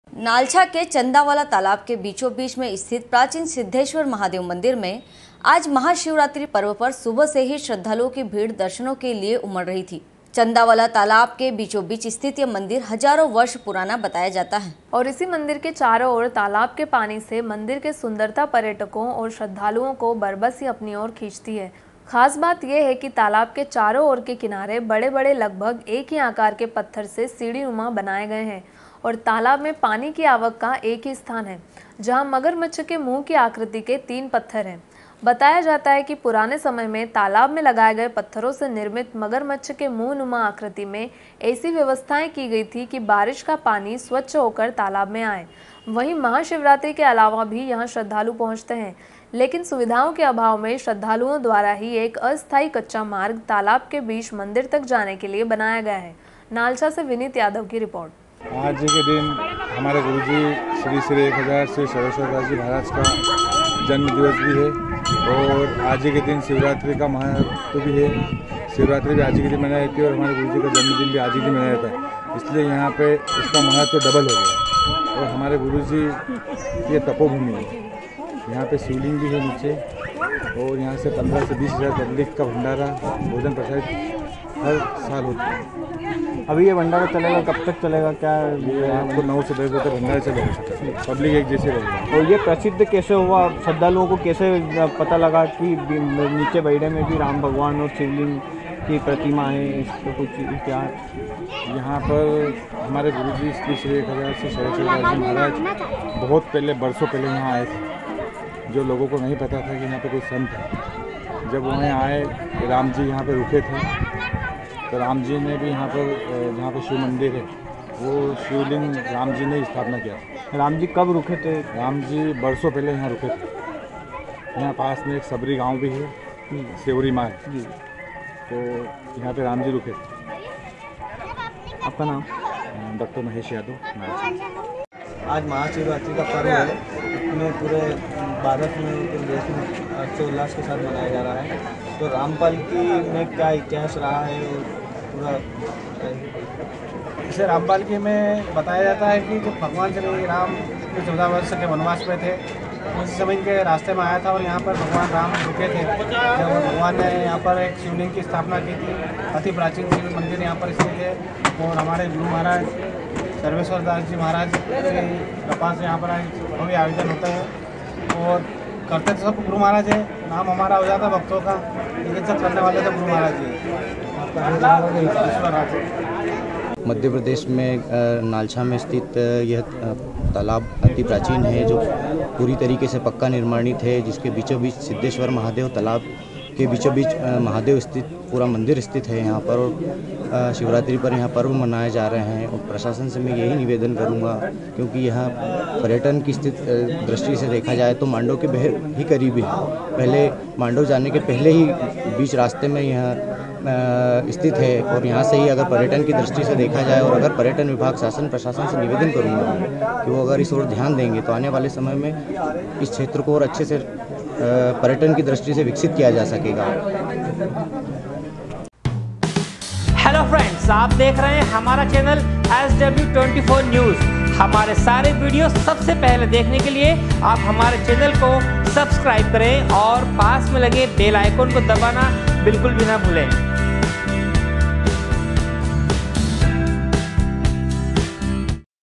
nalcha- प्राचीन सिद्धेश्वर महादेव मंदिर में आज महाशिवरात्रि पर्व पर सुबह से ही
नालछा के चन्दावला तालाब के बीचोबीच में स्थित प्राचीन सिद्धेश्वर महादेव मंदिर में आज महाशिवरात्रि पर्व पर सुबह से ही श्रद्धालुओं की भीड़ दर्शनों के लिए उमड़ रही थी, चन्दावला तालाब के बीचो बीच स्थित यह मंदिर हजारों वर्ष पुराना बताया जाता है। नालछा से विनित यादव की रिर्पोट